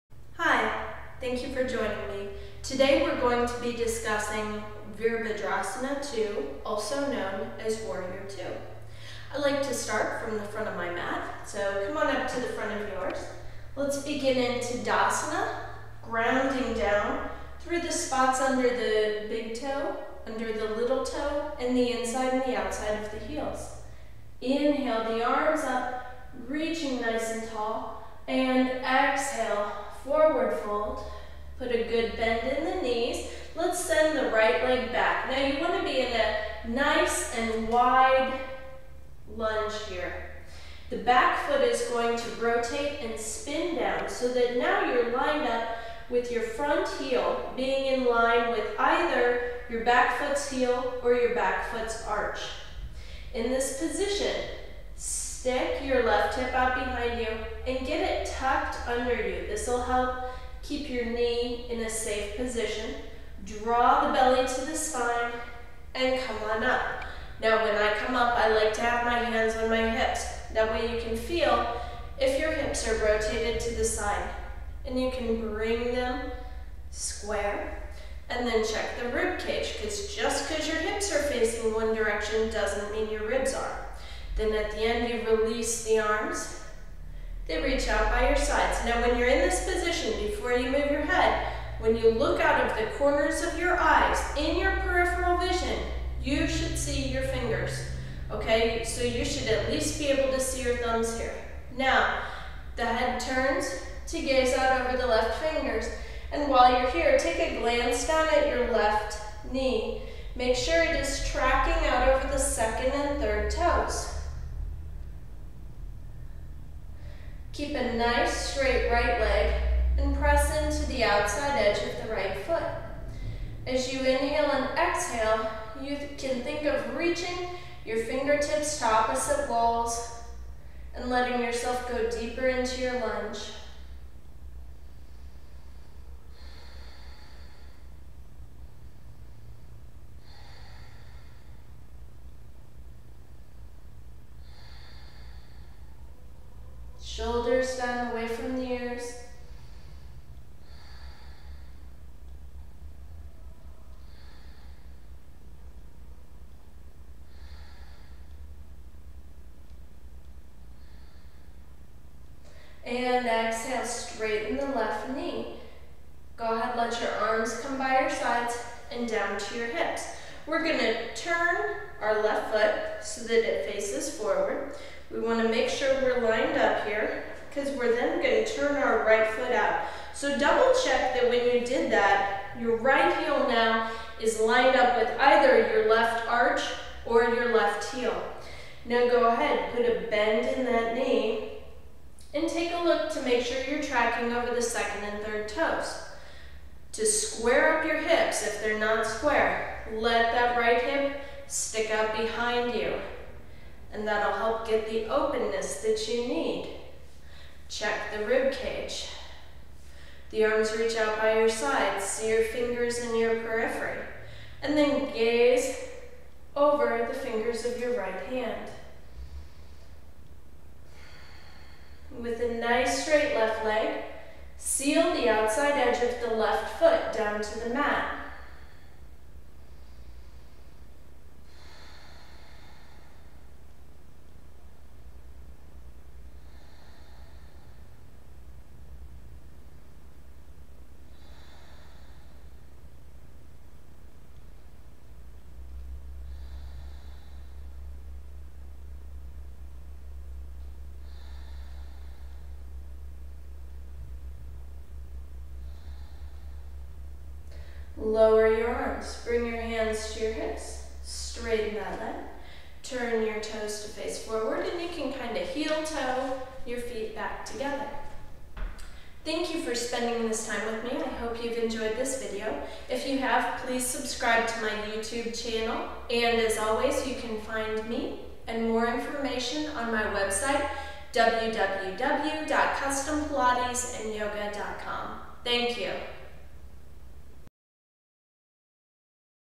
Warrior 2 (Virabhadrasana 2)
Warrior 2 (Virabhadrasana 2) is a wonderful exercise to open the hips and strengthen the core.